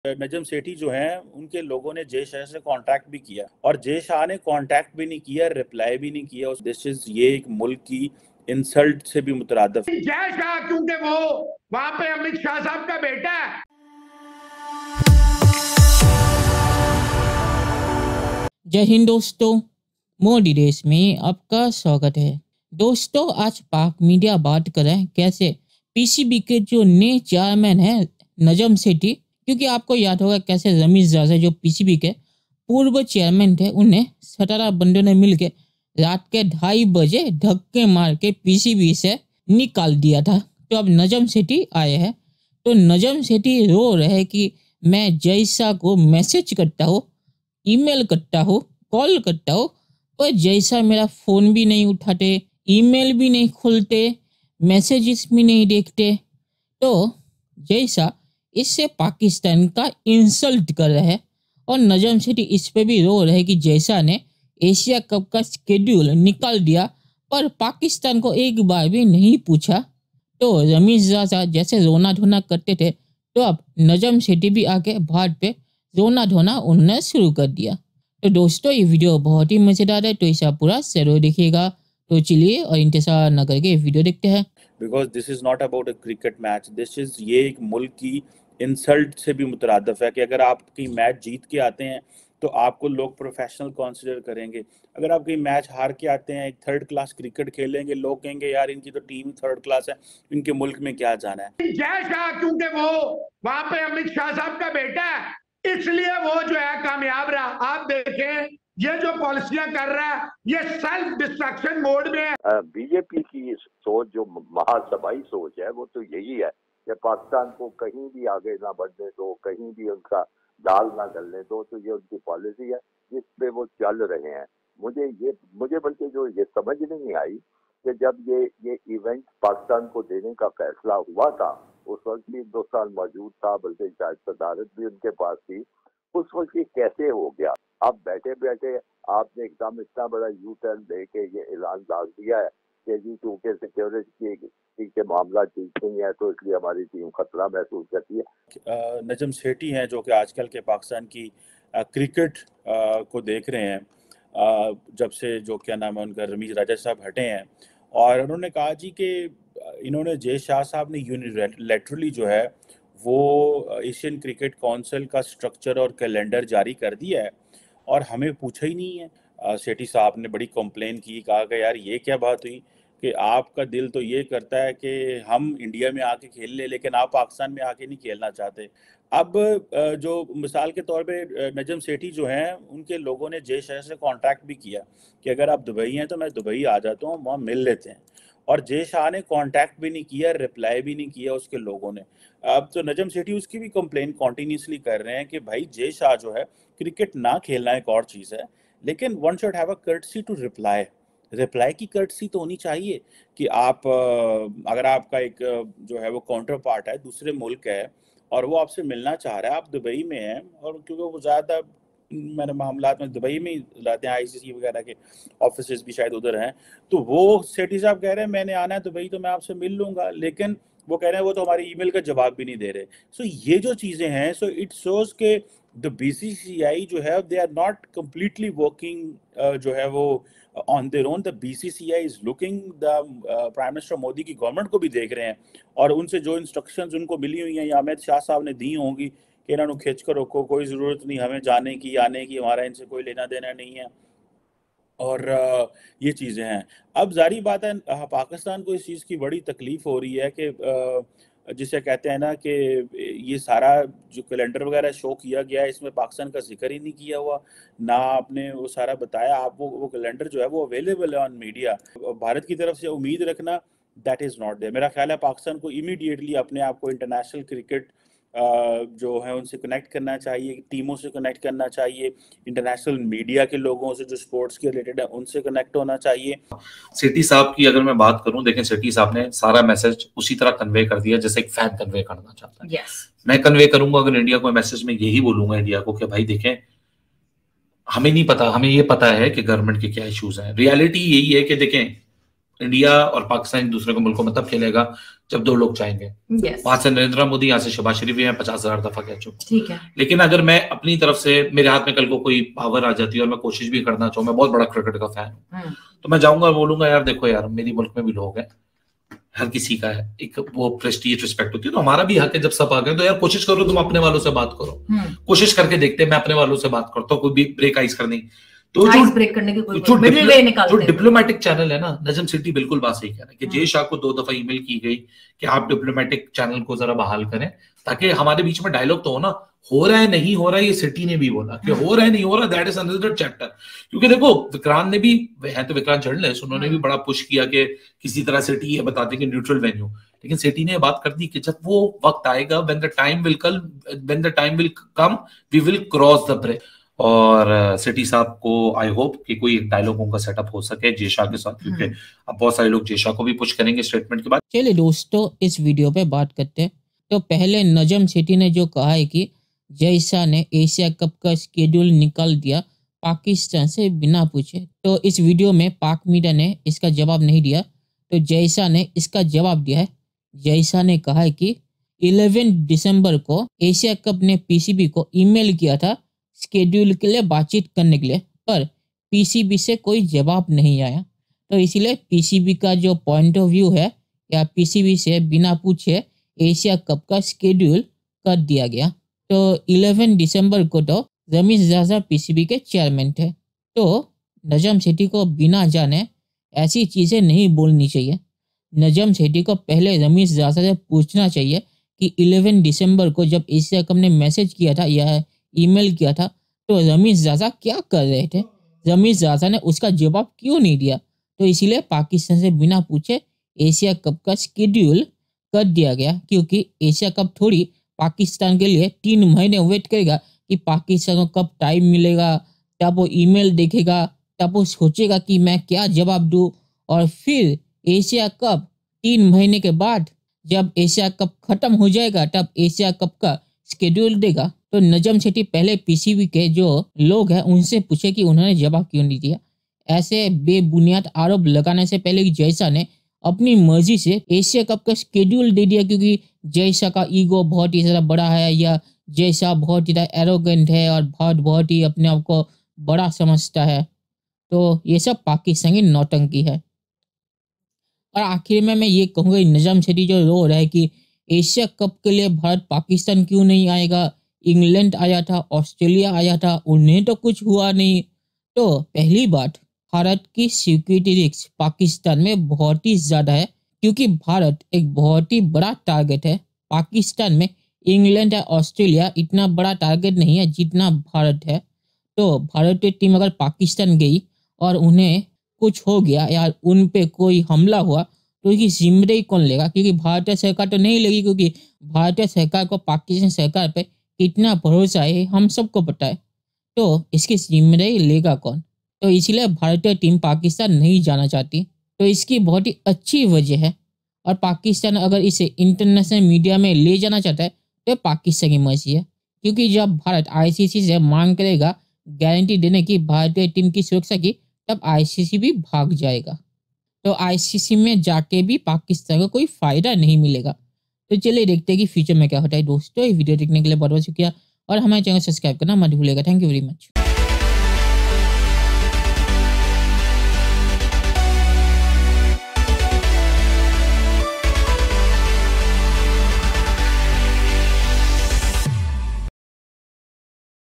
बेइज्जती हो गई Pak Media CRYING That Jay Shah Is IGNORING PCB Calls And Emails (W/Commentary) HINDI
About The Video:  pakistan media talking about how Jay Shah is not picking up PCB Chairman Najam Sethi's Phone Call. Pak media saying this way india isulting pakistan. Pakistani media crying that BCCI won't let indian Cricket team come to Pakistan.